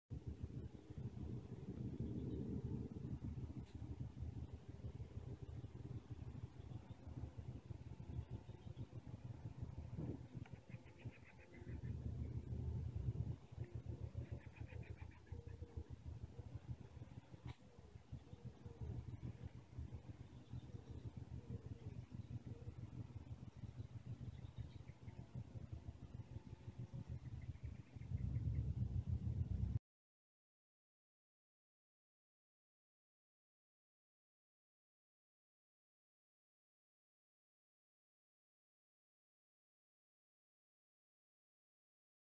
Moths Feeding on a Sponge
A short video showing moths feeding on a damp sponge.

Moths thought to be a "Heart and Club" and a "Heart and Dart" seen in Stevenage on the 1st July 2015.

Sponge wetted with water only. The moths were carefully placed on sponge early in the morning after being caught in a Skinner trap.